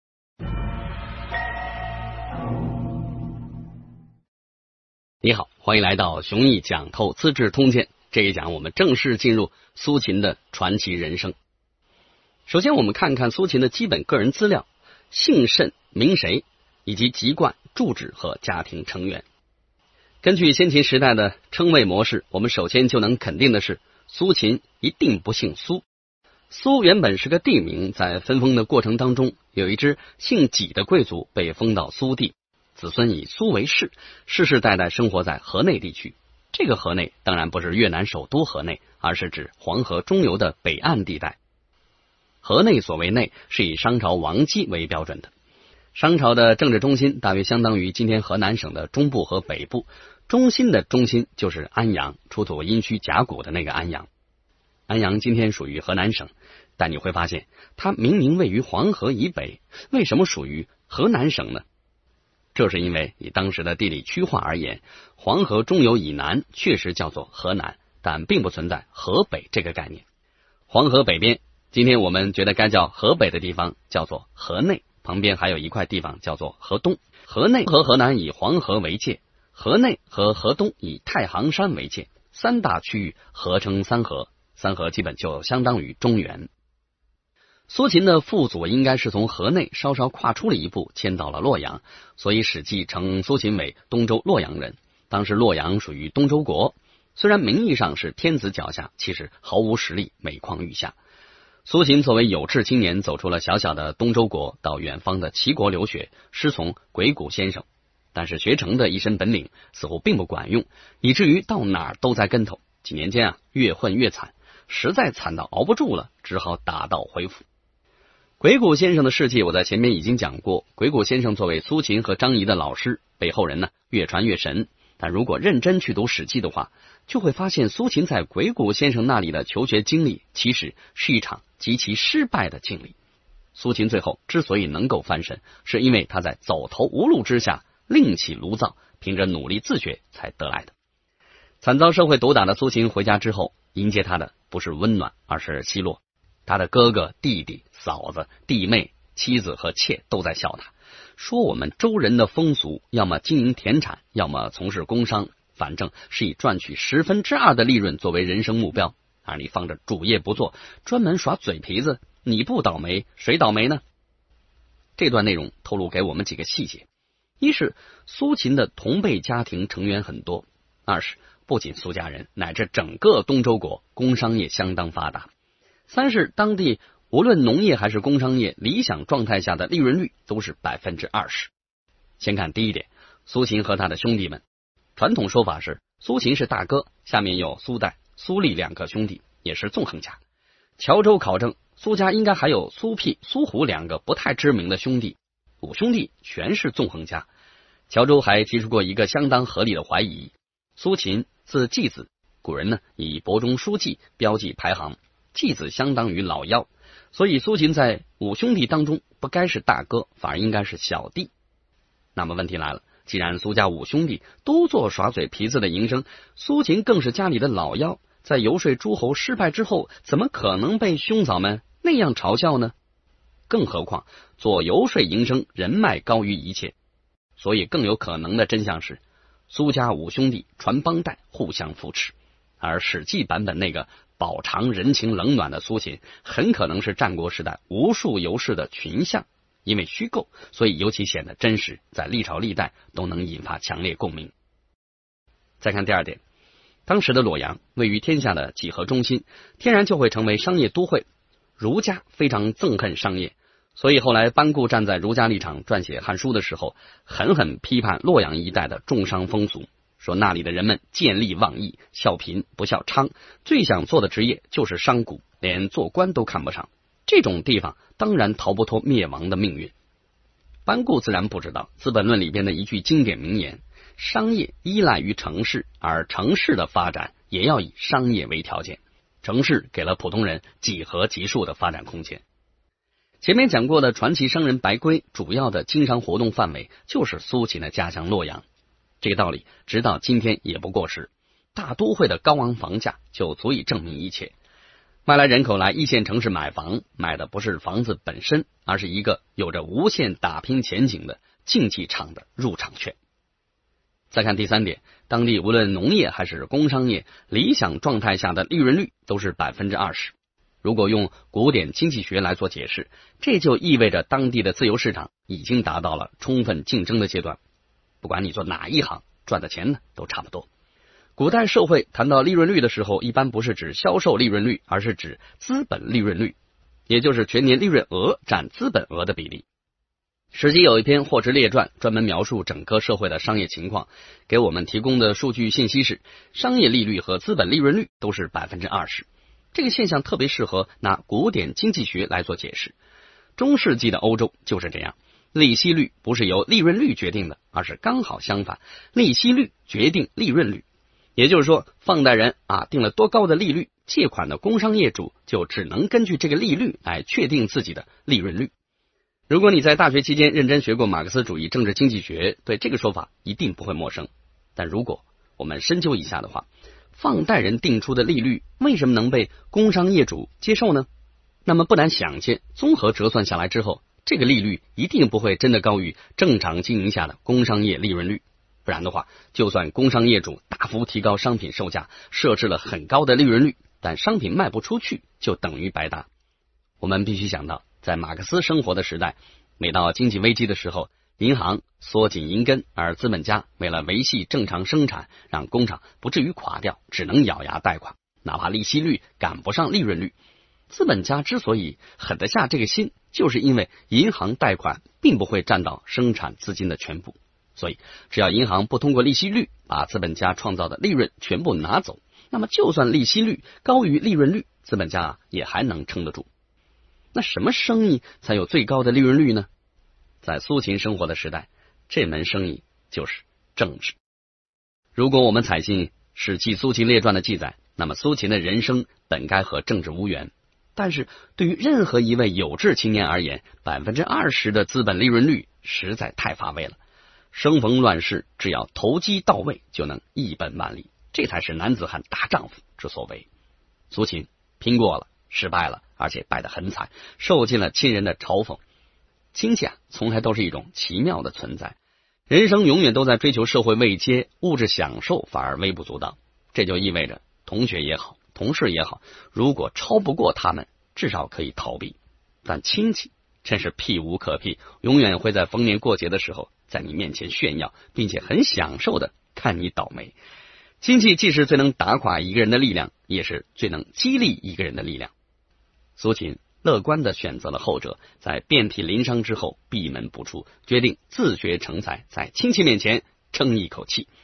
苏秦是怎么跟政治结缘的？ 🔴 讲透资治通鉴（208）
本讲重点：
1. 按照《史记》记载苏秦之所以能够翻身，是因为他在走投无路之下离开鬼谷子另起炉灶，凭借努力自学得来的。
2. 在苏秦生活的时代，无论农业还是工商业，理想状态下的利润率都是20%，政治才是利润率最高的生意。
3. 亲戚，既是最能打垮一个人的力量，也是最能激励一个人的力量。